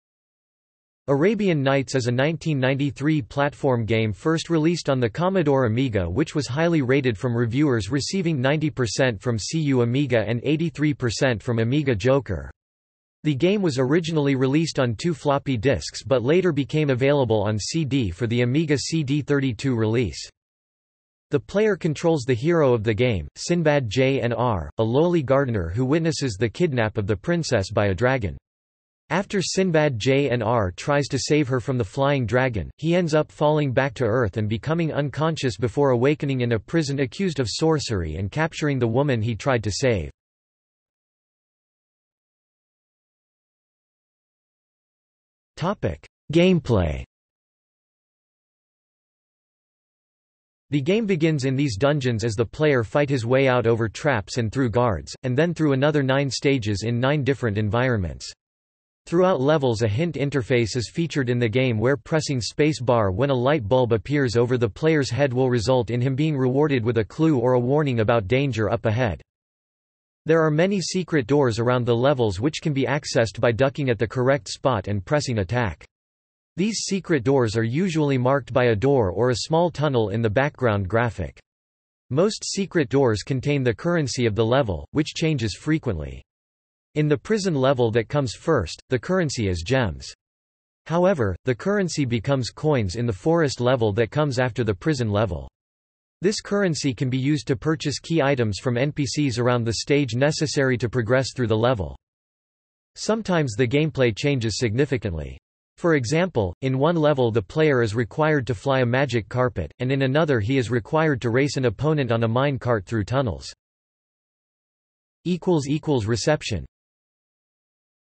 Arabian Nights (video game) | Wikipedia audio article
00:00:42 1 Gameplay

- Socrates

SUMMARY
Arabian Nights is a 1993 platform game first released on the Commodore Amiga which was highly rated from reviewers receiving 90% from CU Amiga and 83% from Amiga Joker. The game was originally released on two floppy disks but later became available on CD for the Amiga CD32 release.
The player controls the hero of the game, Sinbad Jnr, a lowly gardener who witnesses the kidnap of the princess by a dragon. After Sinbad Jnr tries to save her from the flying dragon, he ends up falling back to Earth and becoming unconscious before awakening in a prison accused of sorcery and capturing the woman he tried to save.